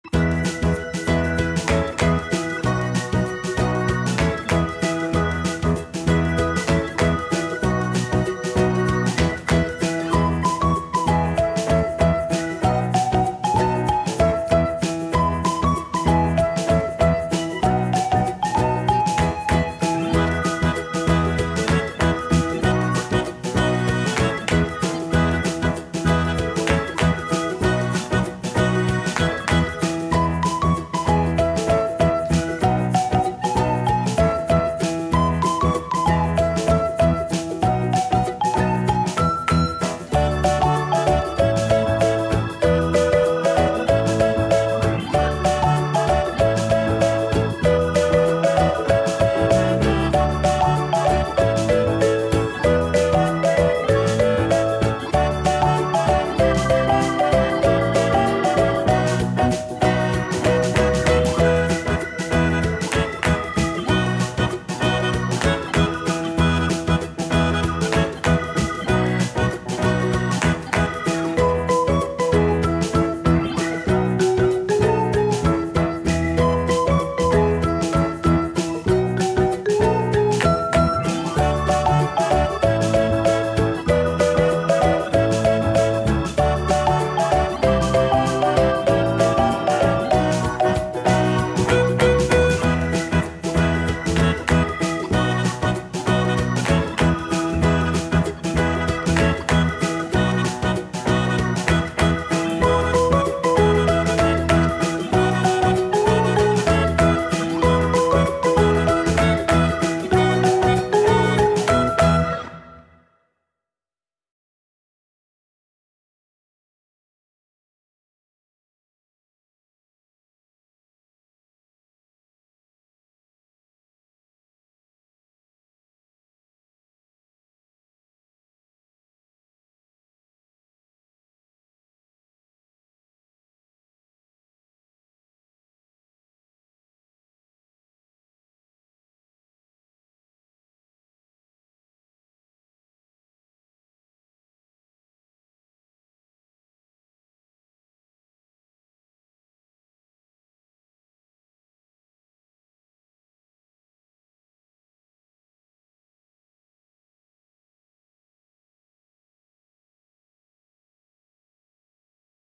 Asian safari Osiris, mâle de 9 semaines réservé
Le bonheur de la chasse au  plumeau!